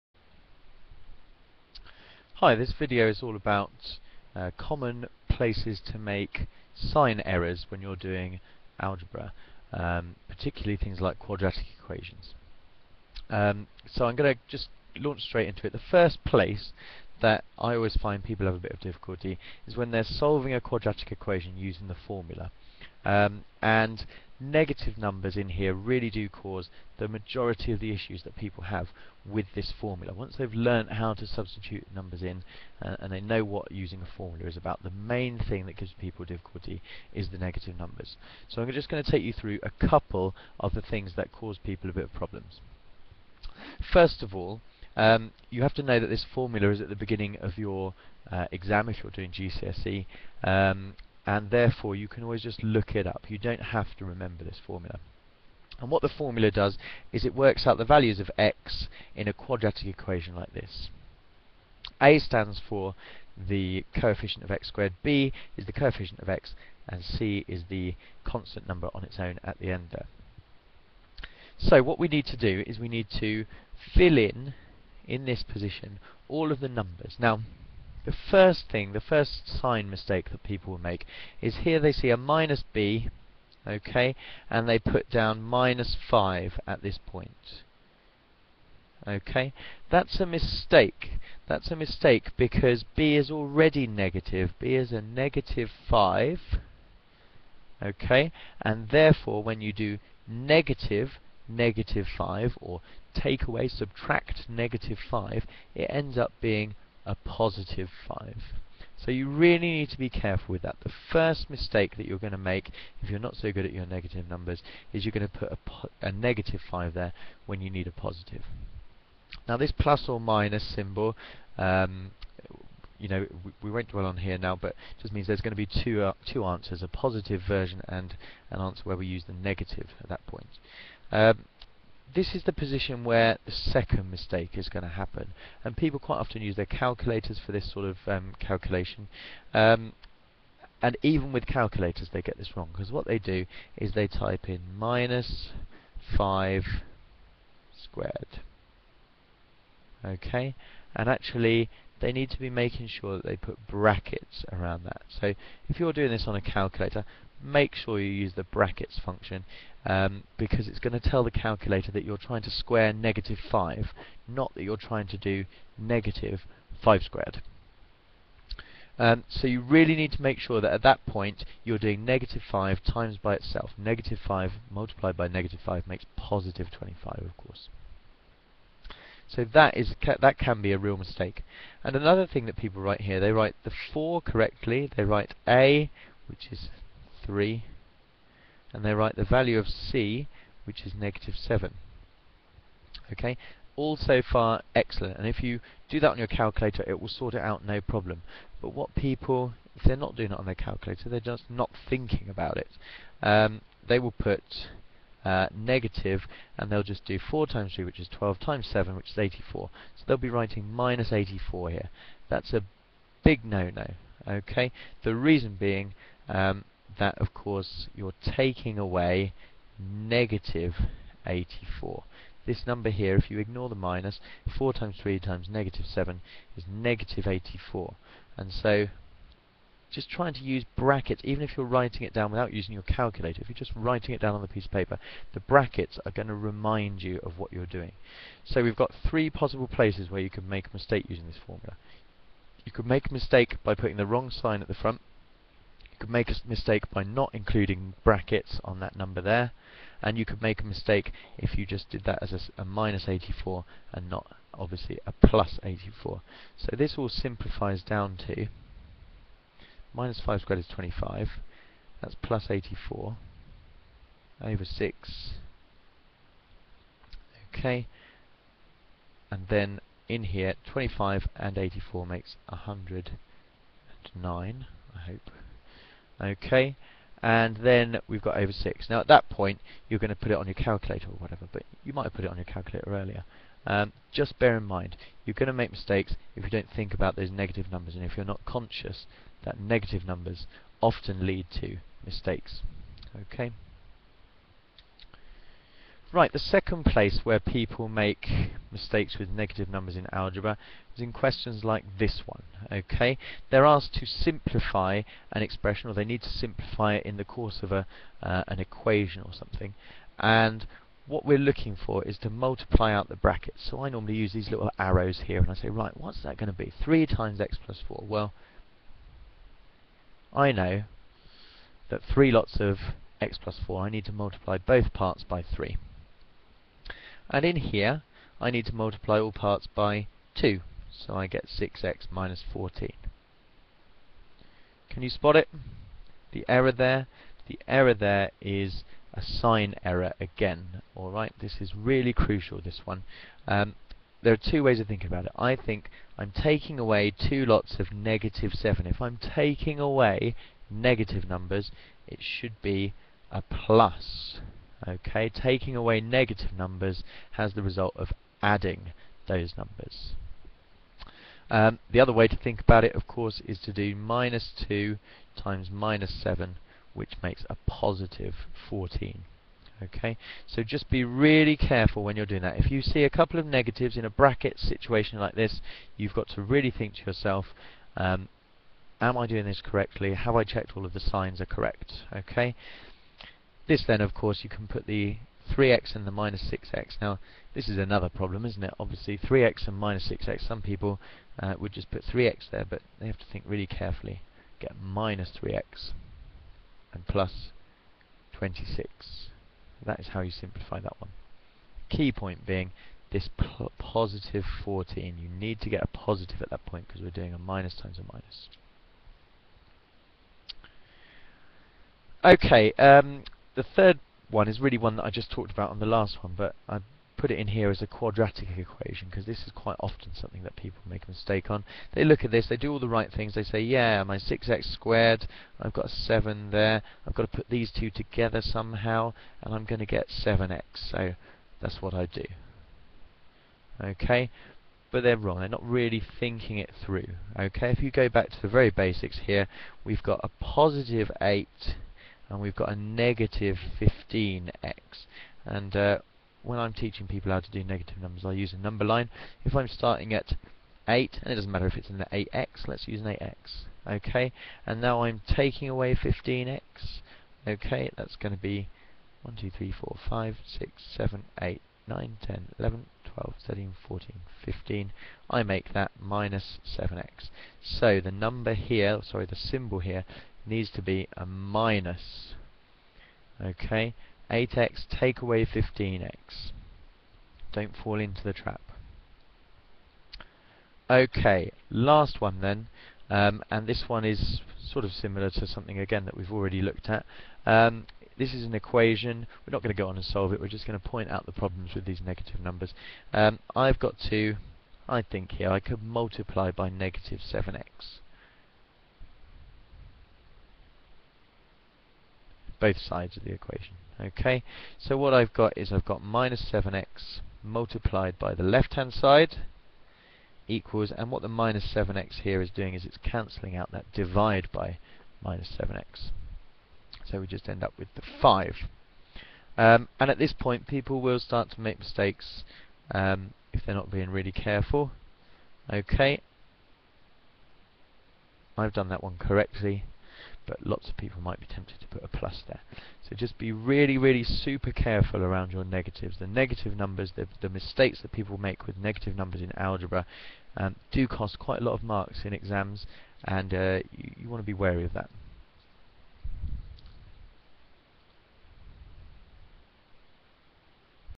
How to - avoid negative number mistakes in algebra
A discussion of four places students find negative numbers impact upon their success in algebra!